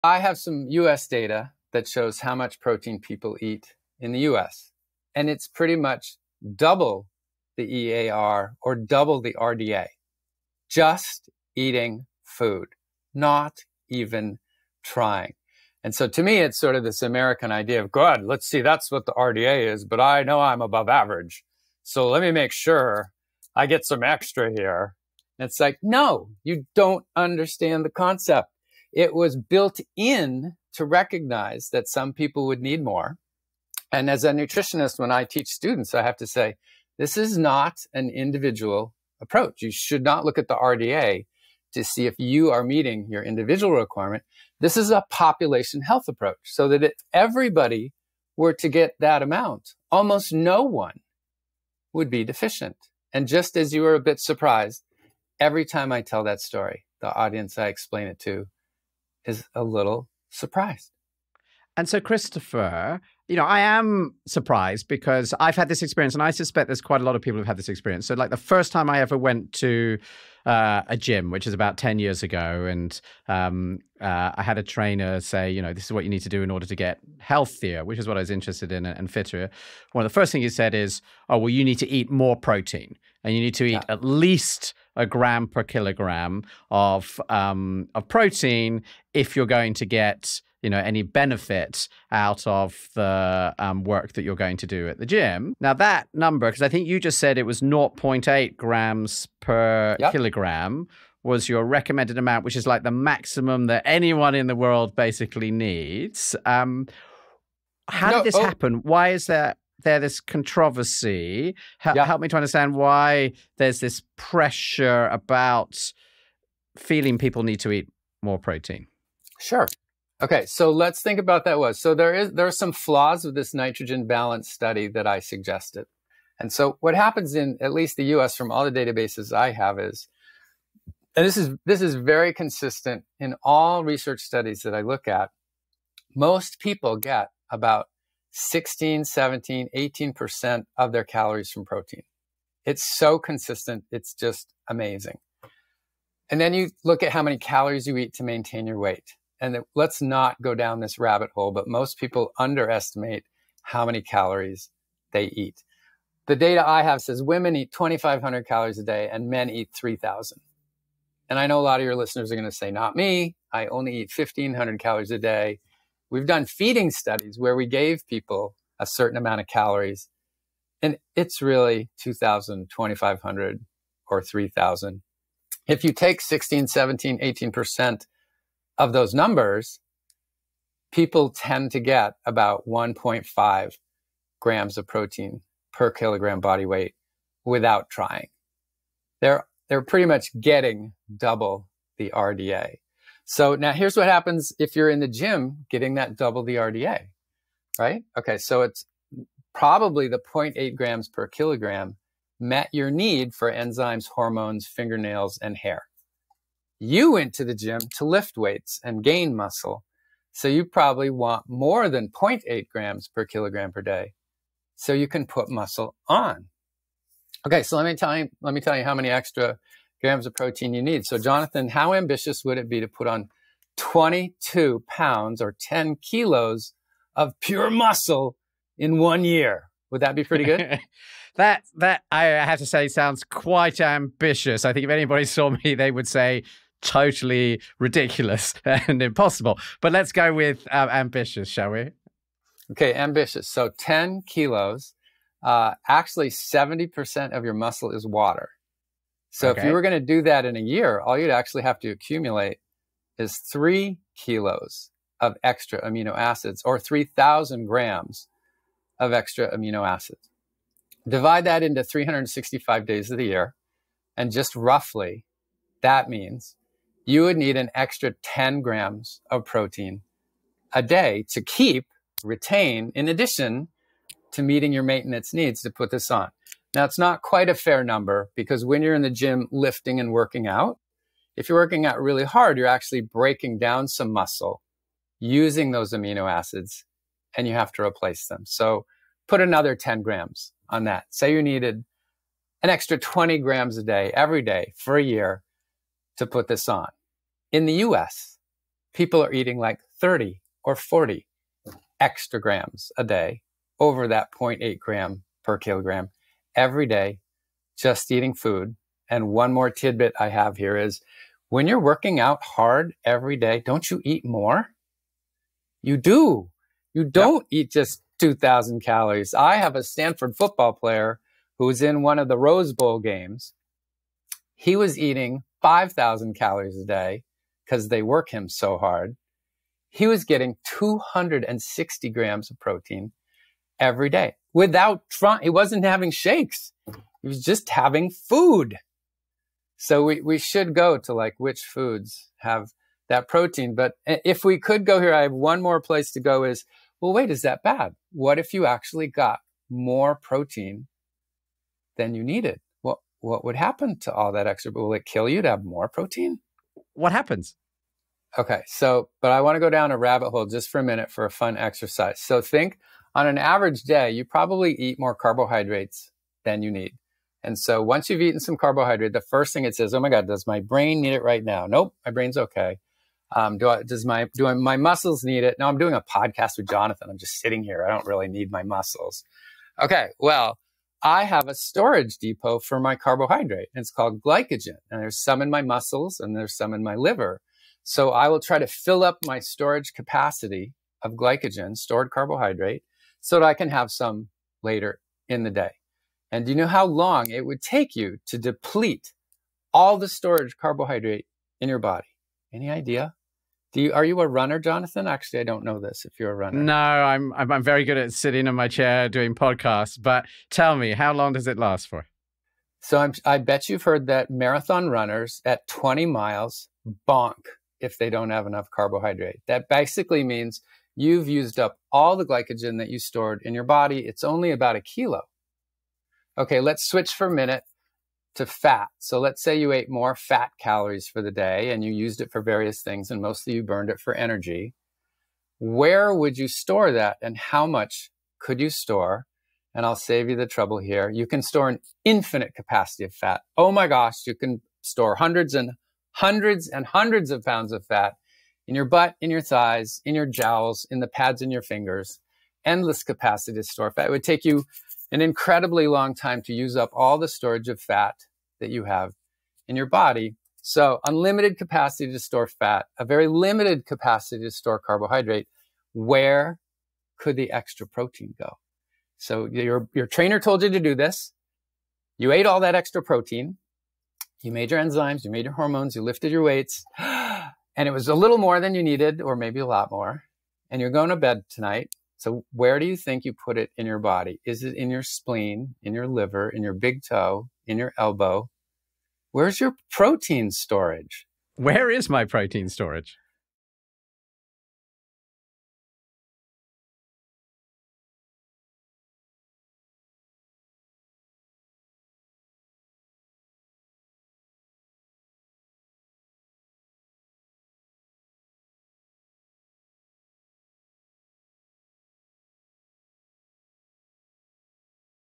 How much protein should I eat? | Christopher Gardener
How much protein are we actually eating and is it really enough?

Jonathan speaks with Christopher Gardner to find out.

Christopher Gardner is a professor at Stanford University and a member of ZOE’s scientific advisory board. He’s pioneering the movement to redefine how we understand the quality of our protein intake.